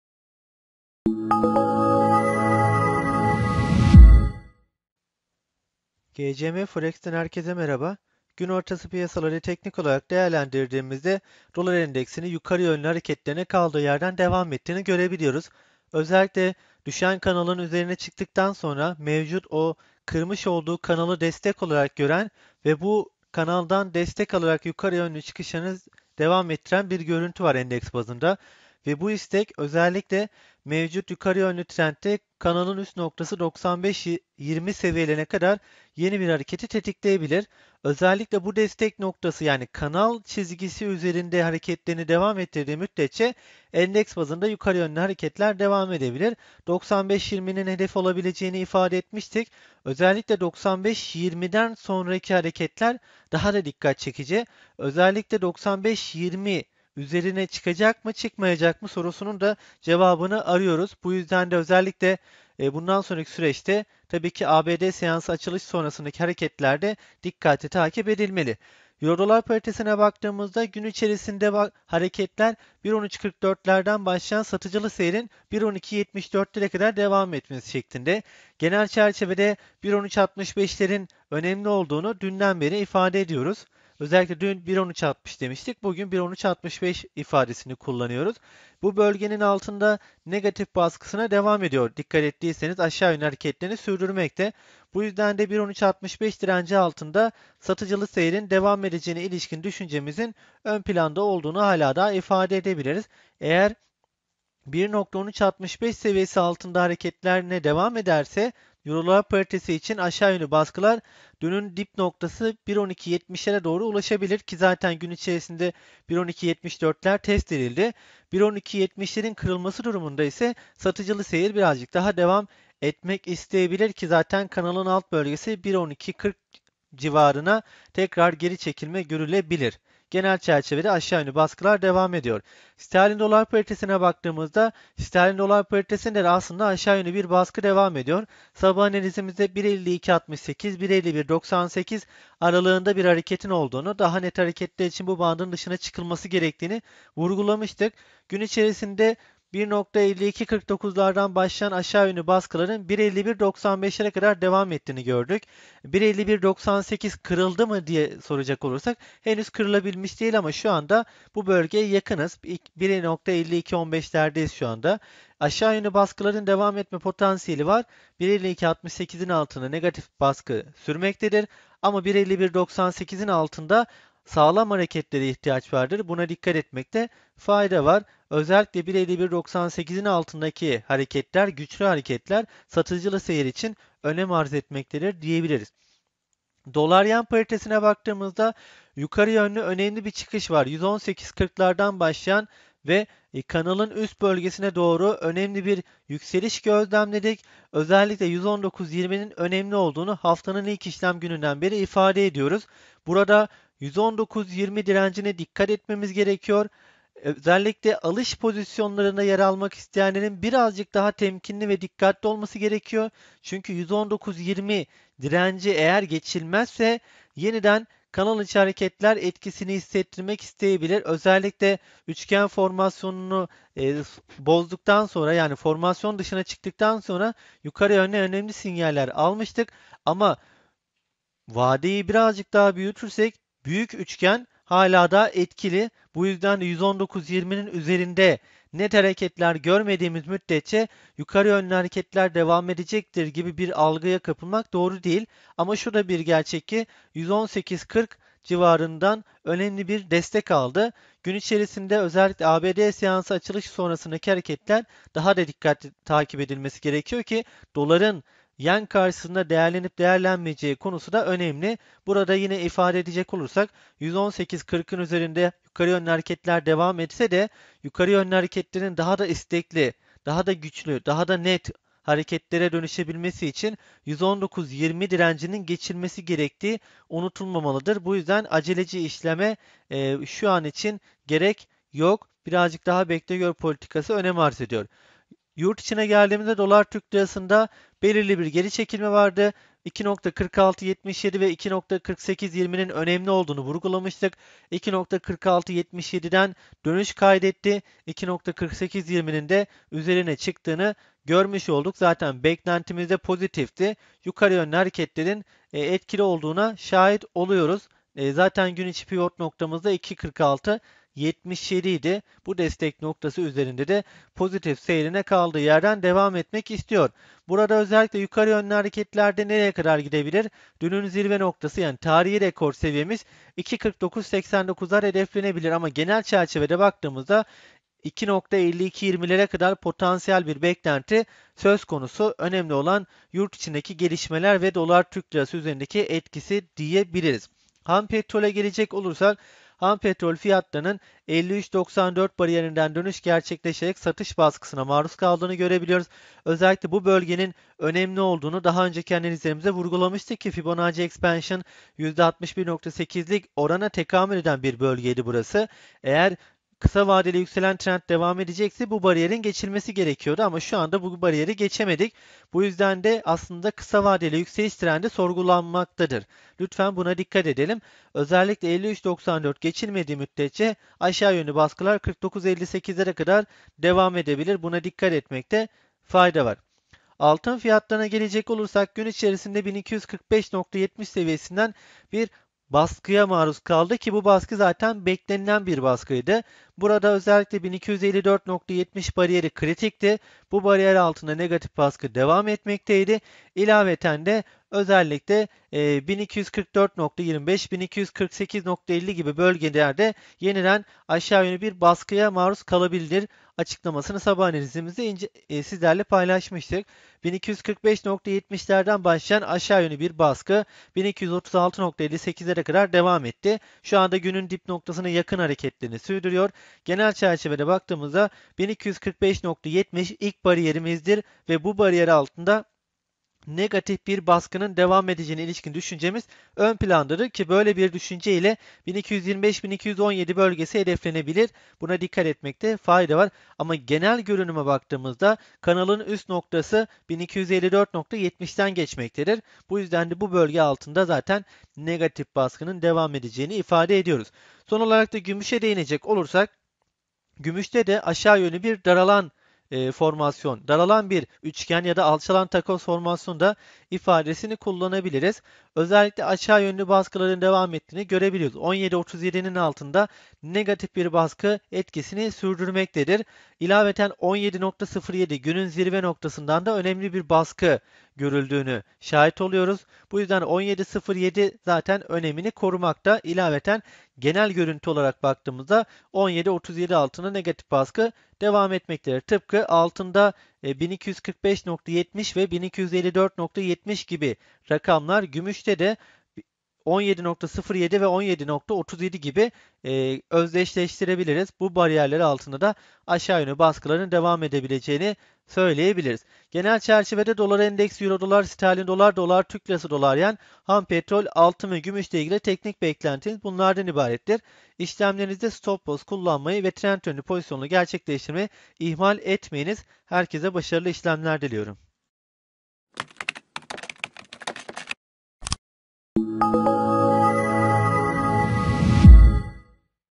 10 ŞUBAT 2015 KUDRET AYYILDIR GUN ORTASI TEKNIK ANALIZ VIDEO KAYDI
GCM Forex Başanalisti Kudret Ayyıldır’ın hazırladığı video çalışmasında, Dolar Endeksi, EURUSD, GBPUSD, USDTRY, USDJPY, Ham Petrol, Altın ve Gümüş gibi finansal varlıkların günün devamındaki teknik beklentiler paylaşılmaktadır